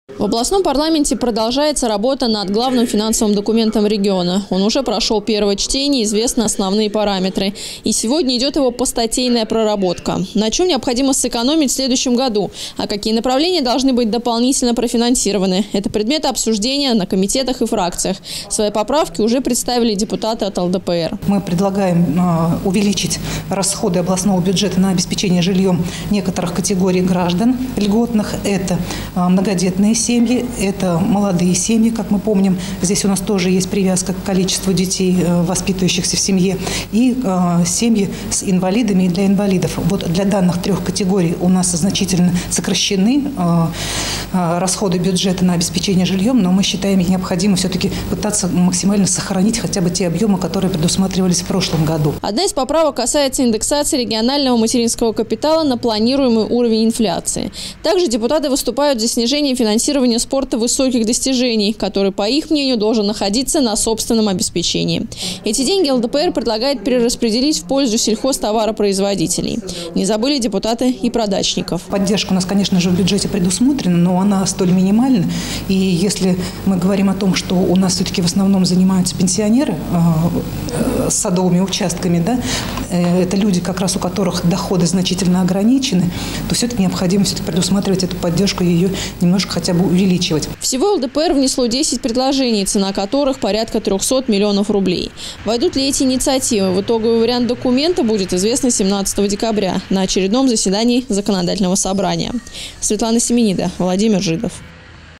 ОБСУЖДЕНИЕ ГЛАВНОГО ФИНАНСОВОГО ДОКУМЕНТА ОБЛАСТИ
Проект областного бюджета продолжает обсуждаться народными избранниками в  Законодательном Собрании. Свои предложения и поправки уже внесли депутаты от ЛДПР. Все подробности далее.